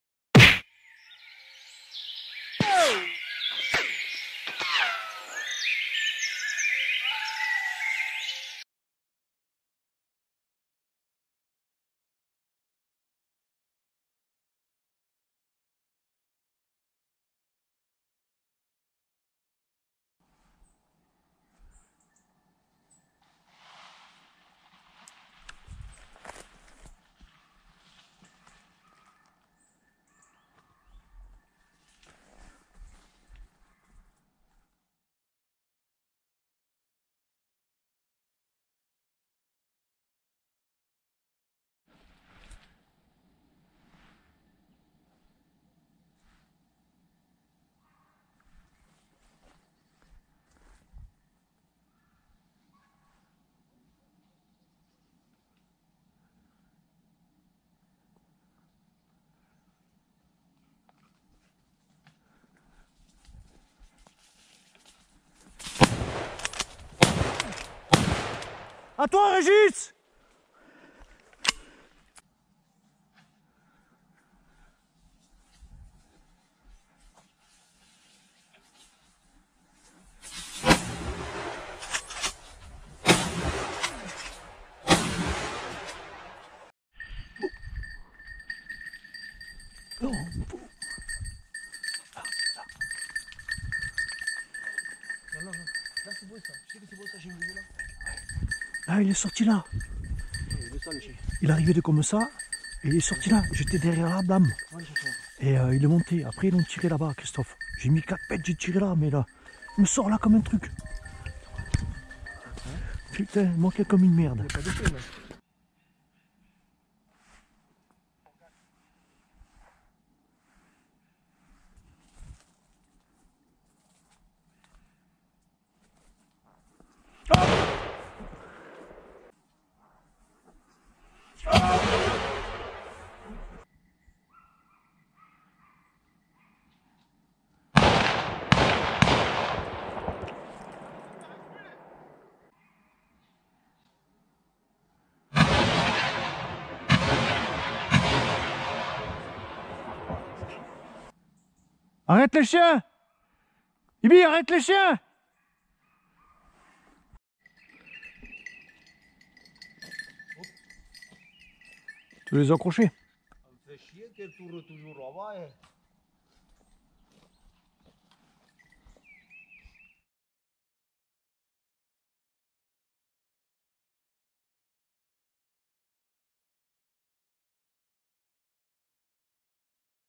Tirs de sangliers à Palaja le 23 12 2022.
Salut les passionés, encore de belles images où les sangleirs profitent de notre maladresse !! C'est noël et cadeau pour eux !
Bon visionnage et à plus dans le bois!